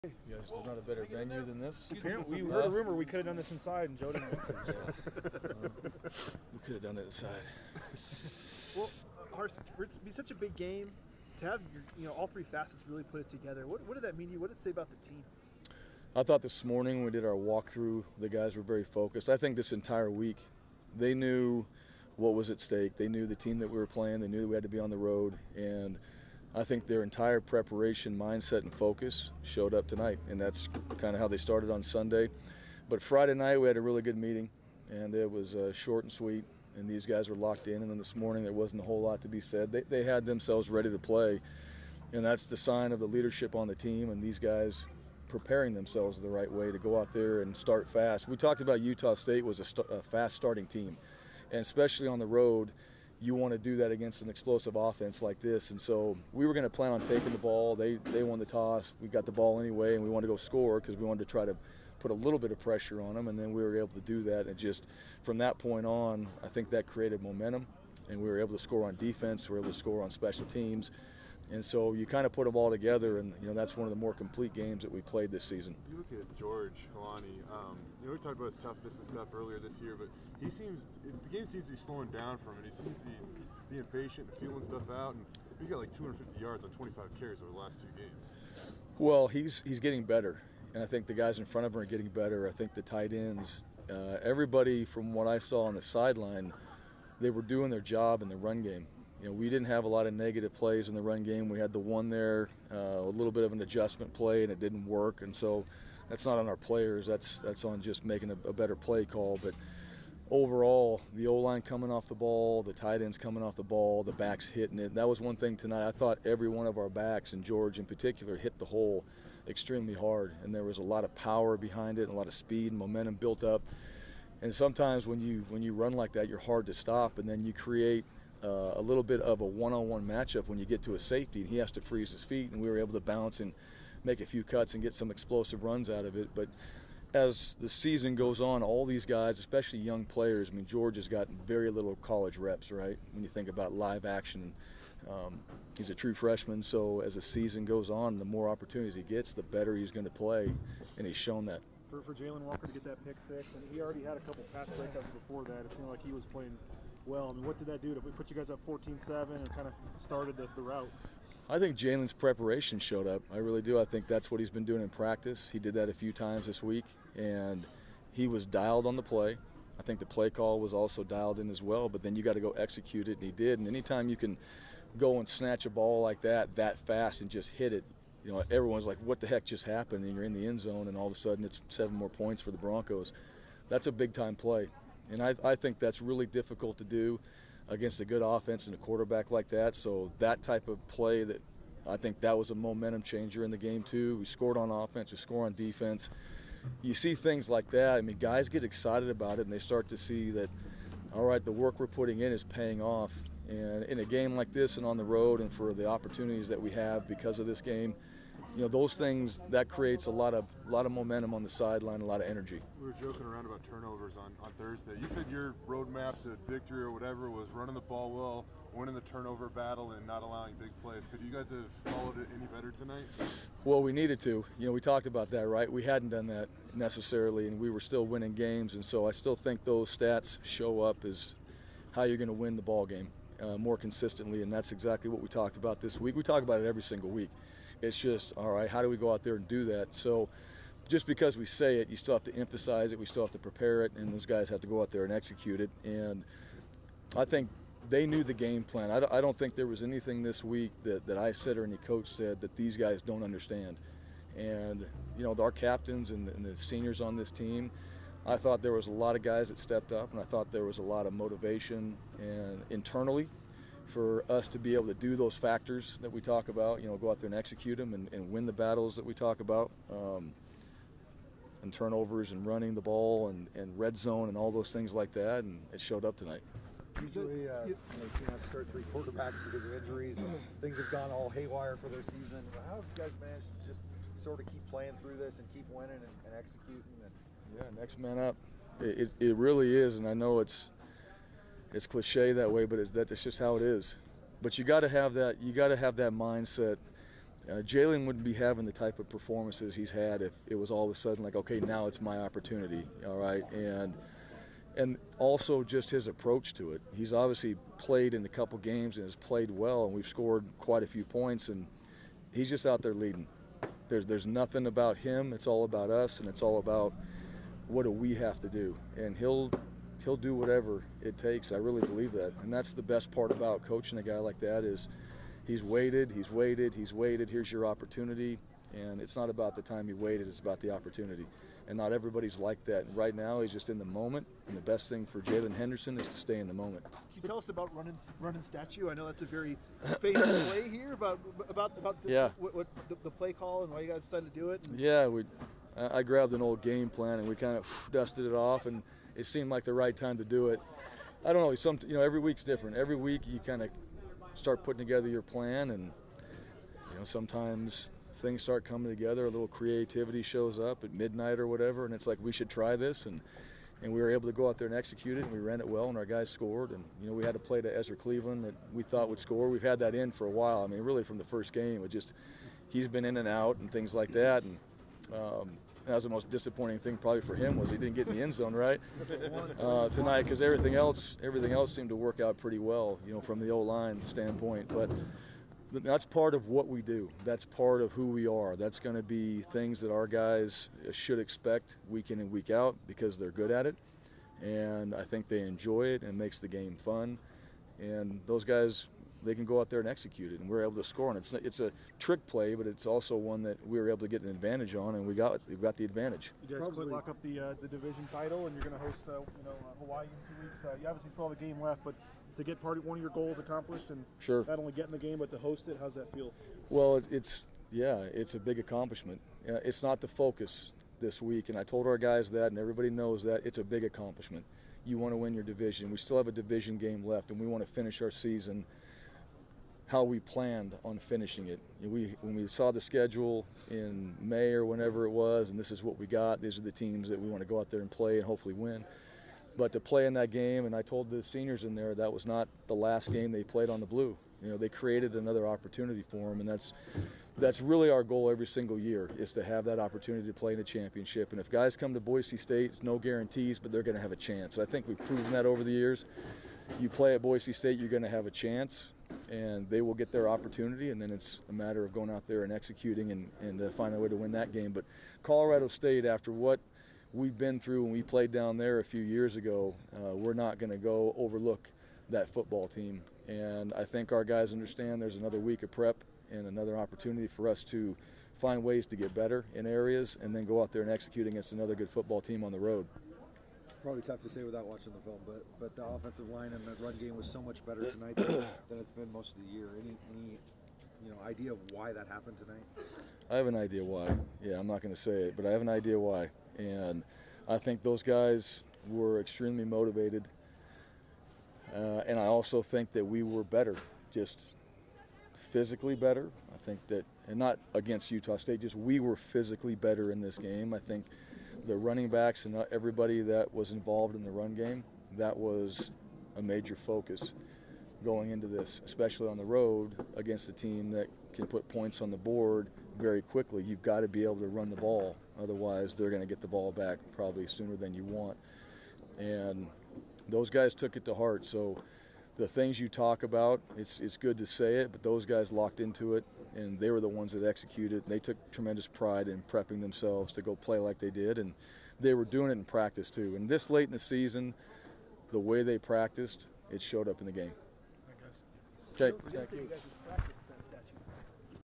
Boise State coach Bryan Harsin breaks down the Broncos' 56-21 win over Utah State
In the most complete game for the Broncos, coach Bryan Harsin discusses the Broncos' 56-21 win over Utah State and how the players prepared to win on the road.